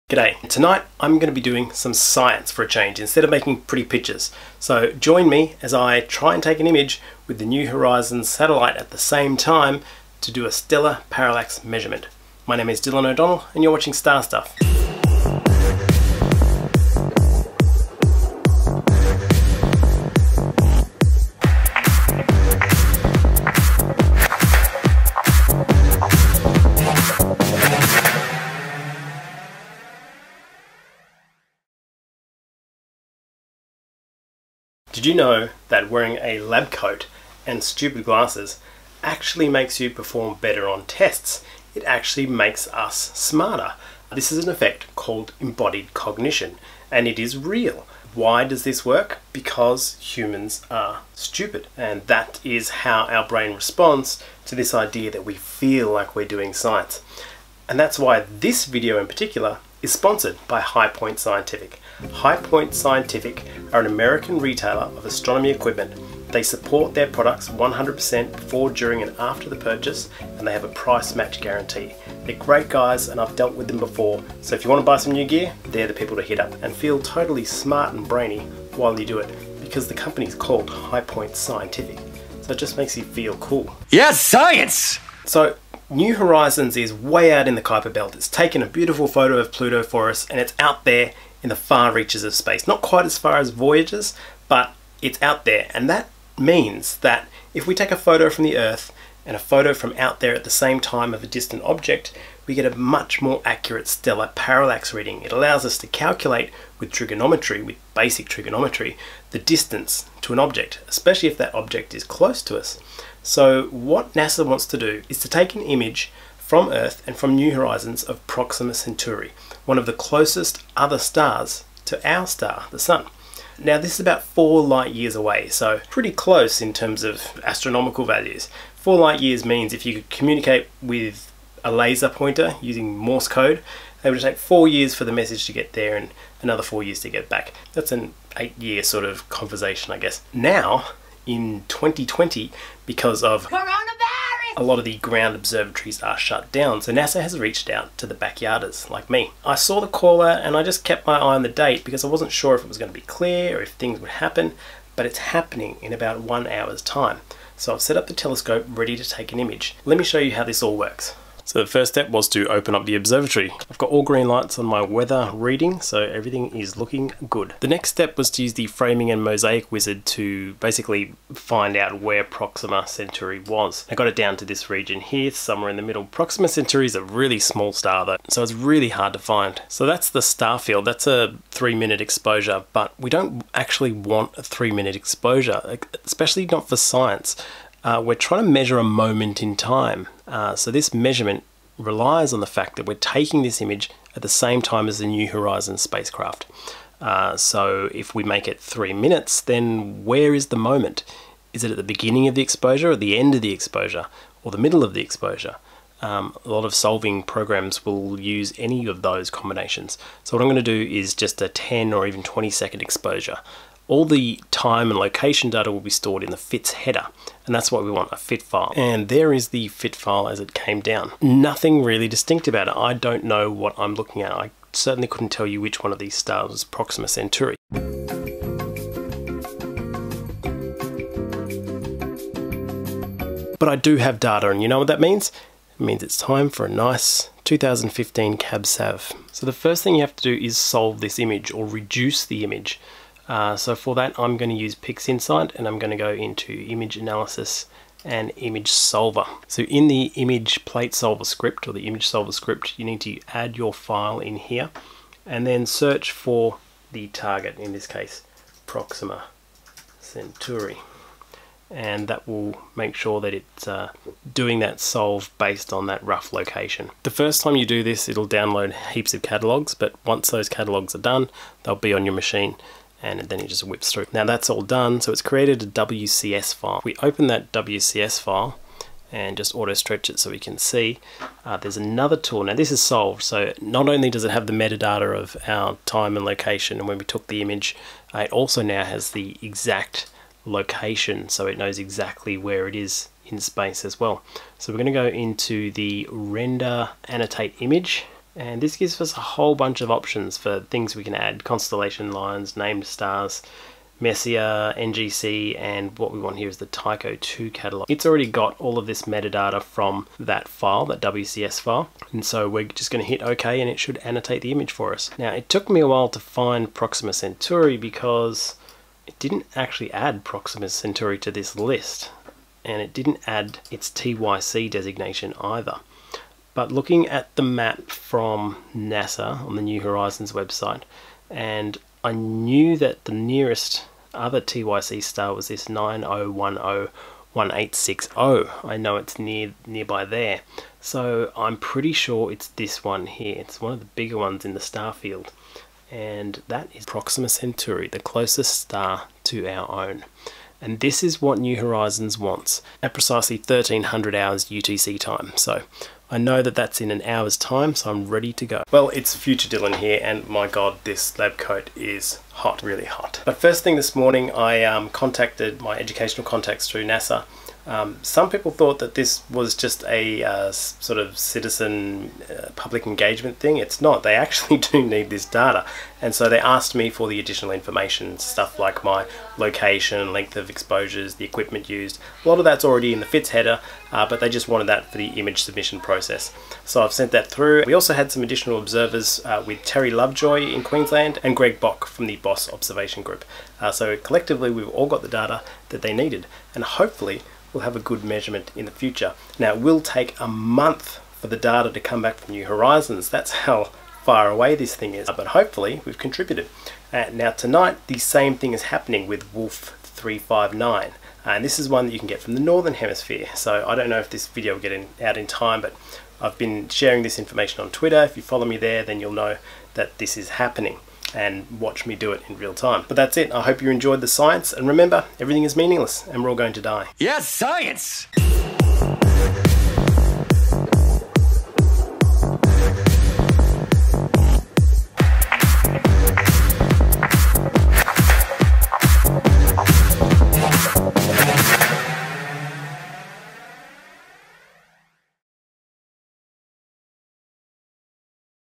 Using Your Telescope for Science - Image Solving and Annotation
I science the sh$# out of my telescope to help NASA work out how far away the aliens are.

EQUIPMENT LIST
Celestron 11" Edge HD Telescope